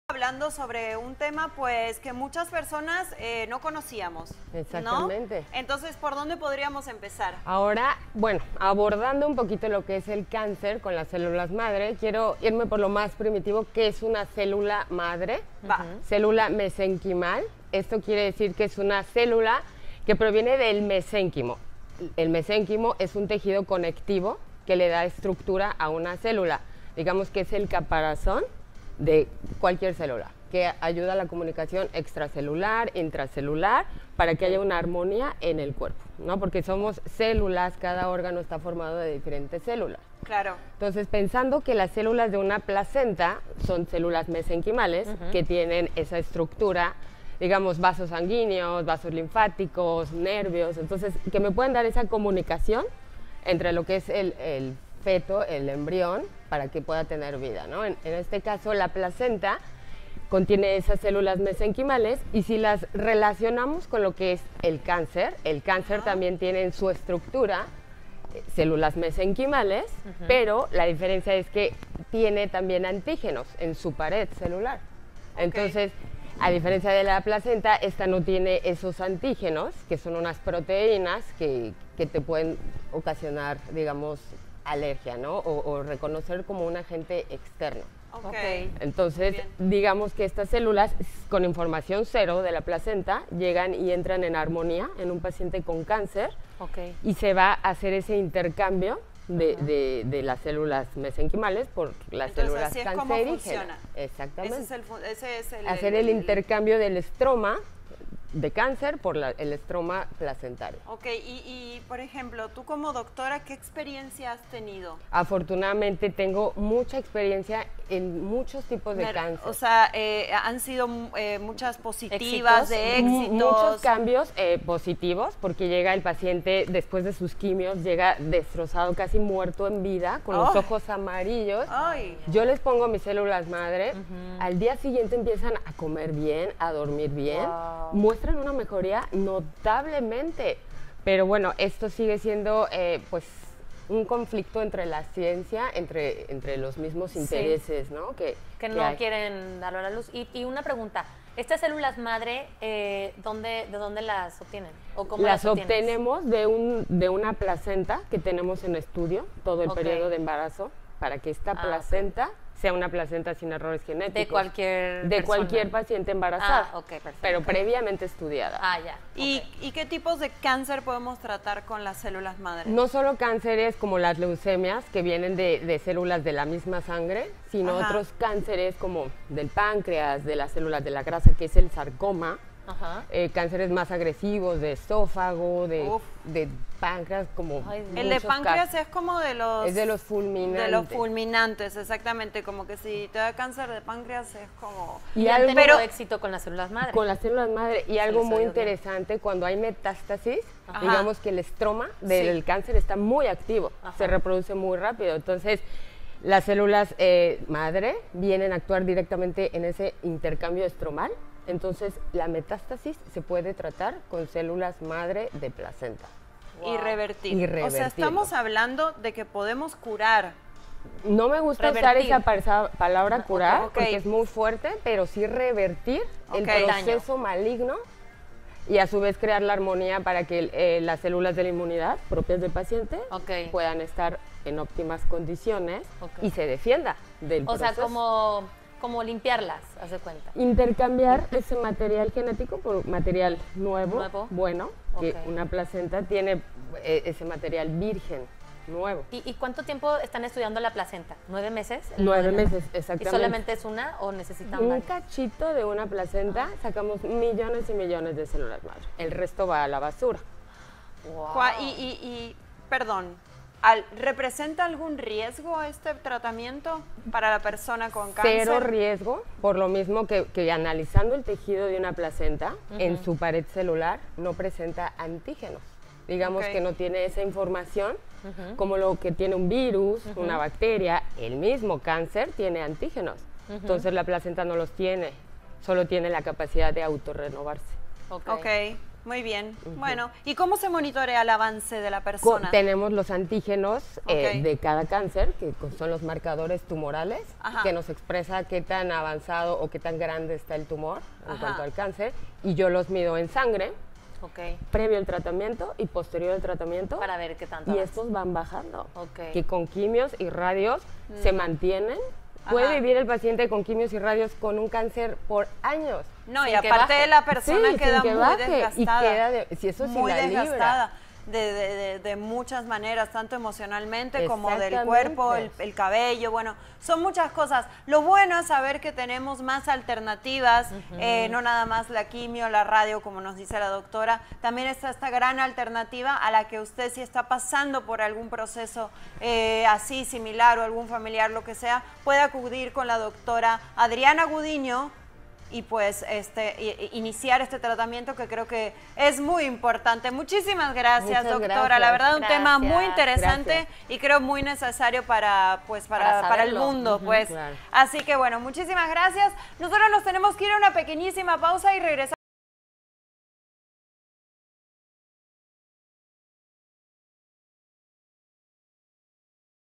Dr Adriana Gudiño's Stem Cell Interview on Televisa Quintana Roo: Cancer Treatment in Cancun, Mexico
Talking about a topic that many of us weren’t familiar with, the discussion of cancer treatment using stem cells opens up a fascinating avenue in modern medicine. To understand this complex subject, it’s essential to start with the basic building blocks: mesenchymal stem cells. These cells originate from the mesenchyme, providing a structural framework that supports both extracellular and intracellular communication, maintaining harmony throughout the body. Since our bodies are composed of various cell types, including those from the placenta, these mesenchymal cells play a crucial role in life. They facilitate vital communication between the fetus and embryo through structures such as blood vessels and lymphatic vessels. In contrast, cancer cells also possess mesenchymal structures, but with one significant difference: they contain antigens in their cell walls, making them potential foreign agents recognized by the immune system.

The unique properties of placental cells make them a promising candidate for cancer treatment. These cells carry no antigenic information, allowing for a harmonious exchange between the cancerous stroma and the healthy placental stroma. By introducing healthy placental mesenchymal stem cells into a cancer patient, a beneficial exchange occurs—where unhealthy cancer cells are replaced with healthy, functional ones. Dr. Adriana Gudiño shared her experiences with patients undergoing this innovative treatment, noting significant improvements after their chemotherapy sessions. Patients who arrived in dire conditions, often exhausted and visibly unwell, experienced remarkable recoveries after receiving placental stem cells, with many regaining their appetite and vitality almost immediately. This raises important questions about the potential of stem cell therapy as a viable alternative to conventional treatments, despite the scientific community's lingering conflicts regarding its acceptance.

The conversation also highlighted the source of these stem cells, which are harvested from carefully studied placentas to ensure they are free of genetic defects throughout the pregnancy. This meticulous process lasts nine months, and just one placenta can yield millions of stem cells for treatment. Notably, Dr. Gudiño emphasized that the treatment poses zero risk to patients, as placental tissue lacks the antigens typically associated with infections or diseases. Monitoring patient progress is achieved through tumor markers—specific antigens in the blood that indicate cancer progression. Unlike chemotherapy and radiation, which often merely maintain cancer markers, stem cell therapy has the potential to reduce these markers, offering hope for a better quality of life. For those facing cancer or supporting a loved one through treatment, exploring the option of stem cell therapy with experts like Dr. Gudiño could provide a transformative path toward recovery.